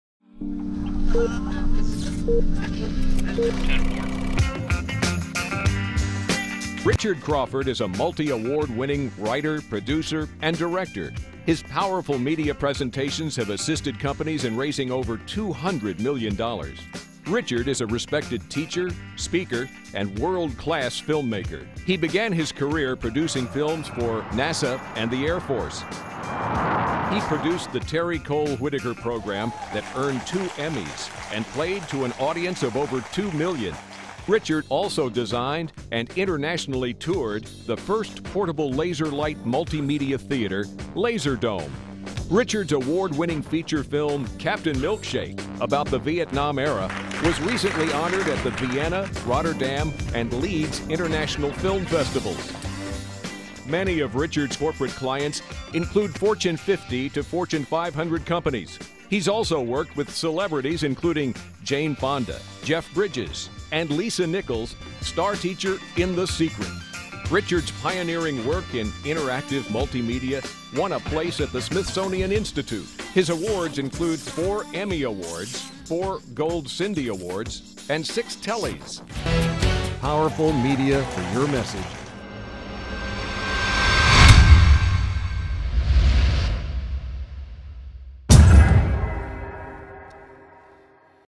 One Productions Demo Reel and introduction of Richard Crawford
A short video featuring Richard Crawford and work by One Productions, a San Diego Video Producton Company